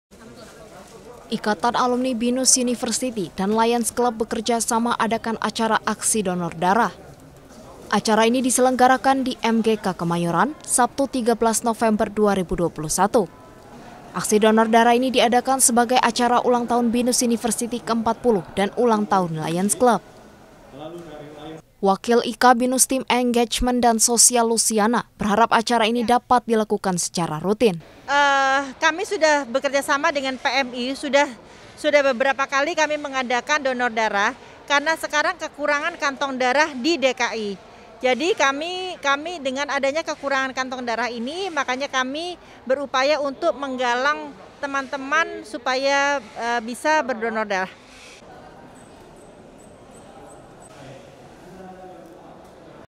Liputan - Lions Club dan IKA BINUS Gelar Aksi Donor Darah
Ikatan Alumni BINUS dan Lions Club adakan aksi donor darah dalam rangka ulang tahun BINUS ke 40 dan ulang tahun Lions Club. Donor darah ini diharapkan dapat membantu masyarakat dan dapat dilakukan secara rutin.